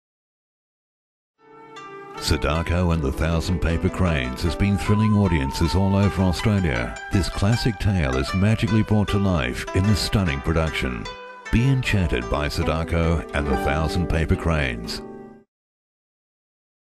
Sadako and the Thousand Paper Cranes, Pilbeam Theatre, September 2-3, 2009
Sadako Sasaki is an ordinary feisty 12 year old who dreams of one day becoming Japans greatest ever runner. Then suddenly she is struck down by a terrible illness. It is caused by radiation from the atom bomb dropped on her home town of Hiroshima when she was only two. But Sadako is full of the spirit of life and never gives up hope. She believes that one day she can again run like the wind . . . if only she can fold a thousand paper cranes.  Based on Eleanor Coerrs best selling novel.

Pilbeam Theatre
Wednesday September 2 at 6.30pm